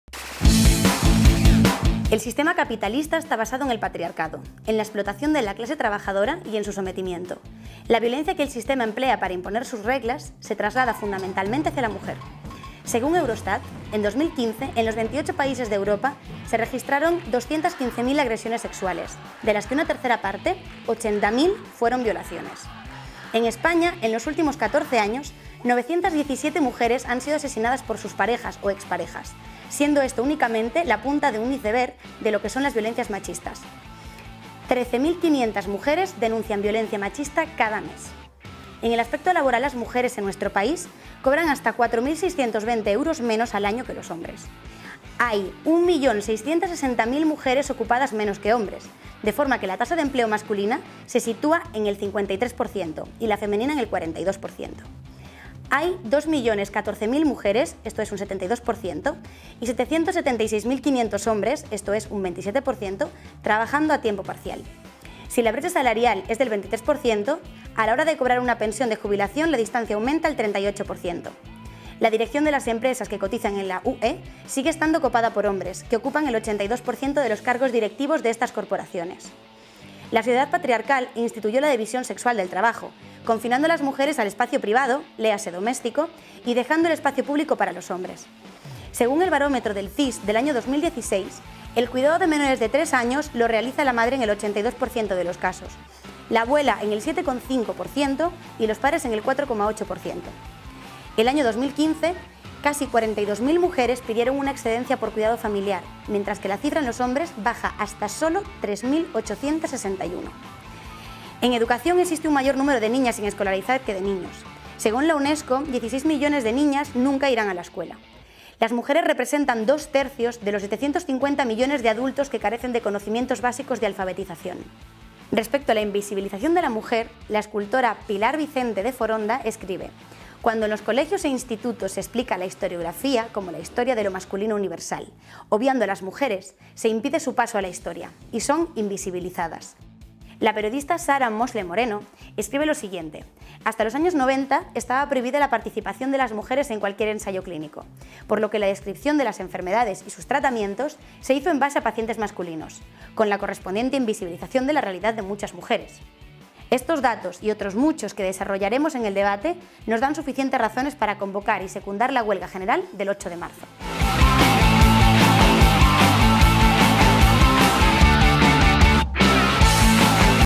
RNtv ¿Por qué es necesaria la Huelga del 8M?
El sistema capitalista está basado en el patriarcado, en la explotación de la clase trabajadora y en su sometimiento. La violencia que el sistema emplea para imponer sus reglas se traslada  fundamentalmente hacia  la mujer.

Según Eurostat, en 2015 en los Veintiocho países de Europa se registraron 215.000 agresiones sexuales, de las que una tercera parte, 80.000 fueron violaciones.

En España en los últimos 14 años, 917 mujeres han sido asesinadas por sus parejas o exparejas, siendo esto unicamente la punta del iucebreg de lo que son las Violencia Machistas 13.500 mujeres denuncian violencia machista cada mes.

En el aspecto laboral las mujeres, en nuestro país, cobran hasta 4.620 euros menos al año que los hombres.

Hay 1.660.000 mujeres ocupadas menos que hombres, de forma que la tasa de empleo masculina se sitúa en el 53 %y la femenina en el 42%.

Hay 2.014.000 mujeres (72%) y 776.500 hombres (27%) trabajando a tiempo parcial,

Si la brecha salarial es del 23% , a la hora de cobrar una pensión de jubilación la distancia aumenta al 38%.

La dirección de las empresas que cotizan en la UE sigue estando copada por hombres, que ocupan el 82% de los cargos directivos de estas corporaciones.

La sociedad patriarcal instituyó la división sexual del trabajo, confinando a las mujeres al espacio privado (léase doméstico) y dejando el espacio público para los hombres.

Según el Barómetro del CIS del año 2016, el cuidado de menores de 3 años lo realiza la madre en el 82% de los casos, la abuela en el 7,5% y los padres en el 4,8%. El año 2015 casi 42.000 mujeres pidieron una excedencia por cuidado familiar, mientras que la cifra en los hombres baja hasta sólo 3.861.

En educación existe un mayor número de niñas sin escolarizar que de niños. Según la Unesco 16 millones de niñas nunca irán a la escuela. Las mujeres representan dos tercios de los 750 millones de adultos que carecen de conocimientos básicos de alfabetización.

Respecto a la invisibilización de la mujer, la escultora Pilar Vicente de Foronda escribe “Cuando en los colegios e institutos se explica la historiografía como la historia de lo masculino universal, obviando a las mujeres se impide su paso a la historia y son invisibilizadas. Con lo cual, de forma discriminatoria, se pierde la oportunidad de narrar a los hombres y mujeres del futuro de cómo media humanidad mantenía sojuzgada a la otra media.”

La periodista Sara Mosleh Moreno escribe lo siguiente Hasta los años 90 estaba prohibida la participación de las mujeres en cualquier ensayo clínico, por lo que la descripción de las enfermedades y sus tratamientos se hizo en base a pacientes masculinos, con la correspondiente invisibilización de la realidad de muchas mujeres.

Estos datos y otros muchos que desarrollaremos en el debate nos dan suficientes razones para convocar y secundar la huelga general del 8 de Marzo.